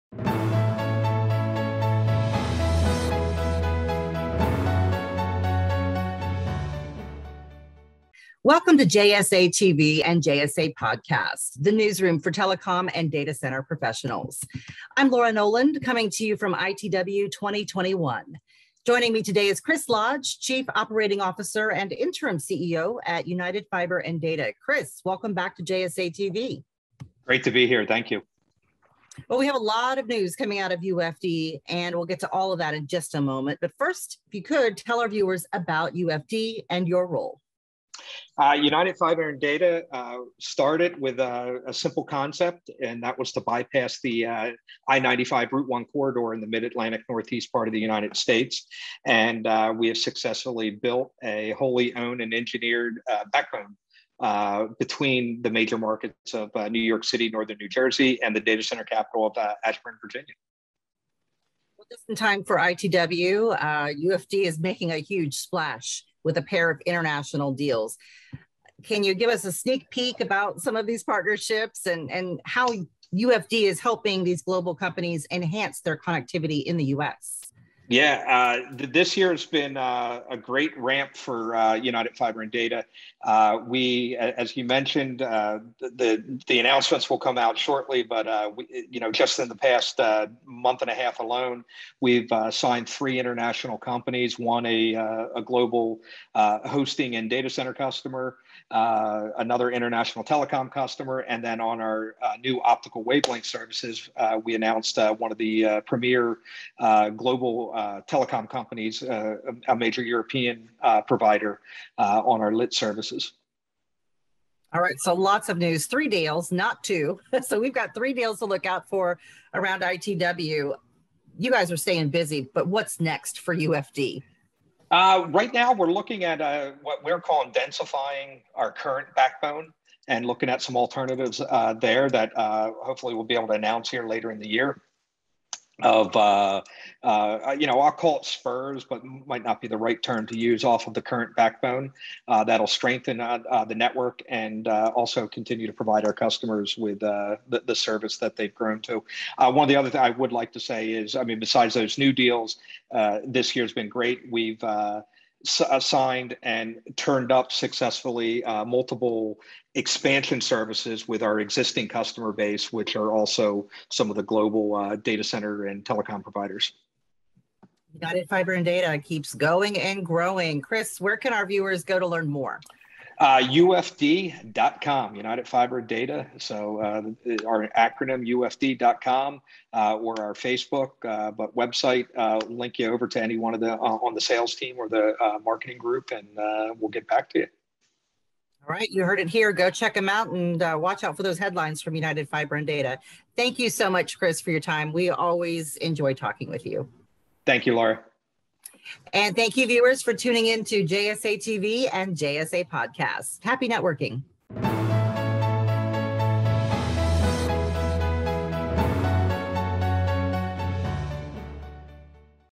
ITW2021 | Big Wins for United Fiber & Data with 3 Major International Deals
Big news out of United Fiber & Data (UFD)! Christopher Lodge, Chief Operating Officer and Interim CEO of UFD sits down with JSA to talk about the company’s three recent and major international deals. UFD provides high-capacity, low latency, geographically diverse fiber optic network solutions in the northeastern United States. The global telecom companies partnered with UFD to enhance their diversity and capacity outside the Mid-Atlantic I-95 corridor. UFD’s route spans from northern New Jersey to the data center capital of the world, Ashburn, Virginia.